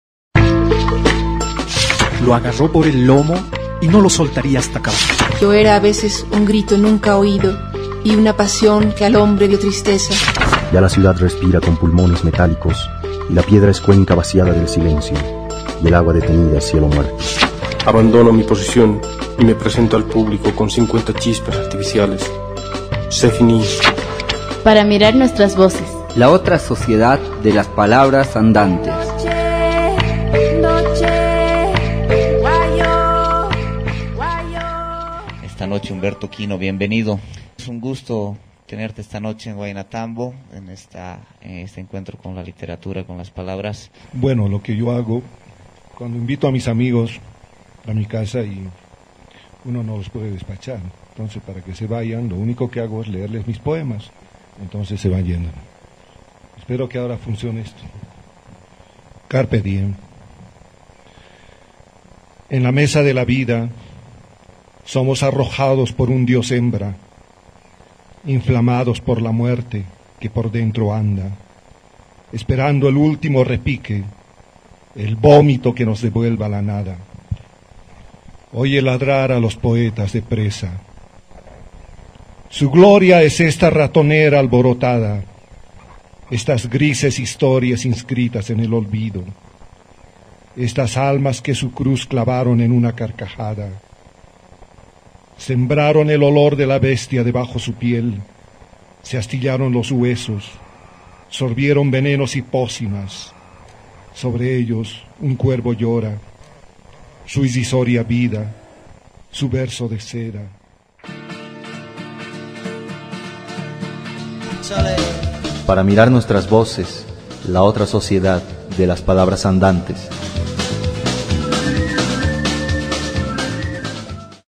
Humberto Quino lee dos poemas de "Coitus ergo sum"
Nuestro espacio de literatura, poesía, relatos y otros.
Humberto Quino Márquez (La Paz – 1950), es pastor de ovejas descarriadas, la demencia es su corona, corcovado y alegre, también es poeta, conocido por su acido humor y sus posturas anarquistas. Ávido lector y tenedor de libros. En su ser pesan varios poemarios, fue director de las revistas: Humus, Papel Higiénico, Dador, El Sueño de la Razón, Tiro al Aire, La Torre de los Locos, y fundador de Camarada Mauser.
✅Radio Wayna Tambo 101.8 FM (El Alto – La Paz)
✅Radio Sipas Tambo 92.2 FM (Sucre)
✅Radio Yembatirenda 100.6 FM (Tarija)
Wayna Tambo - Red de la Diversidad.